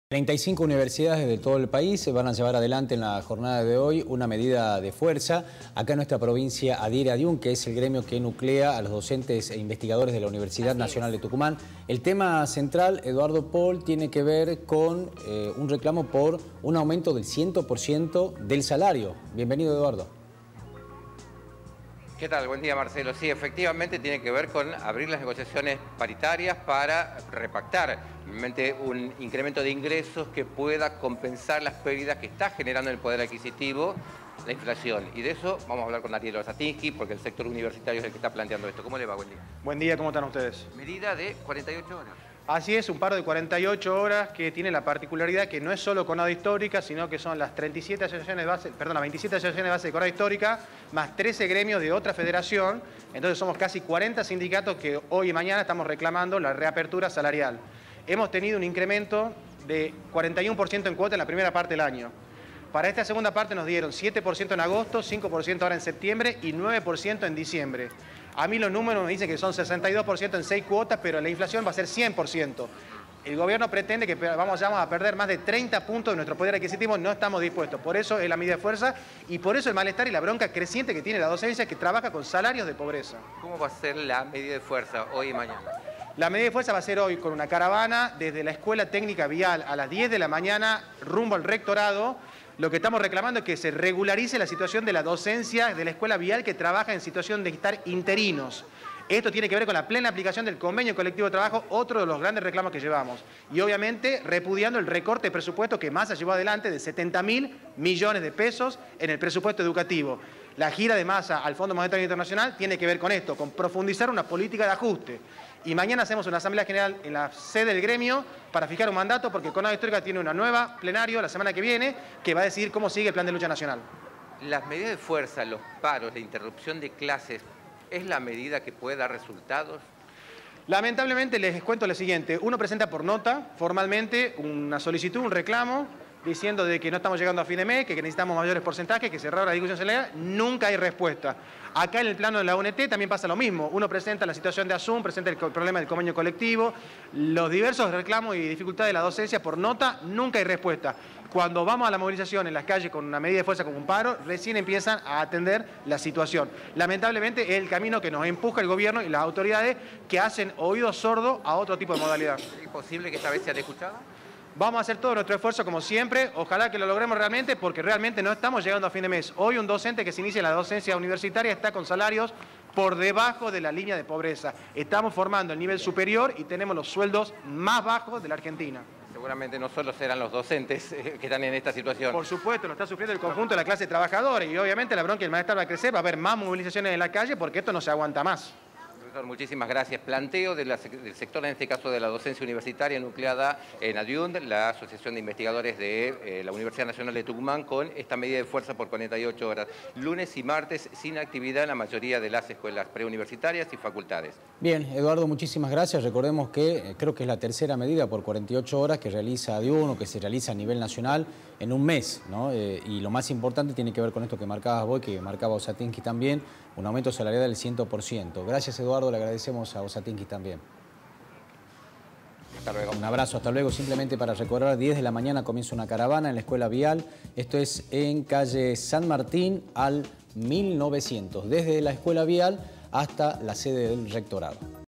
Paro de ADIUNT: los docentes universitarios paran hoy y mañana
Hoy marcharán desde la Escuela Técnica Vial en San Martín al 1.900 hasta el Rectorado.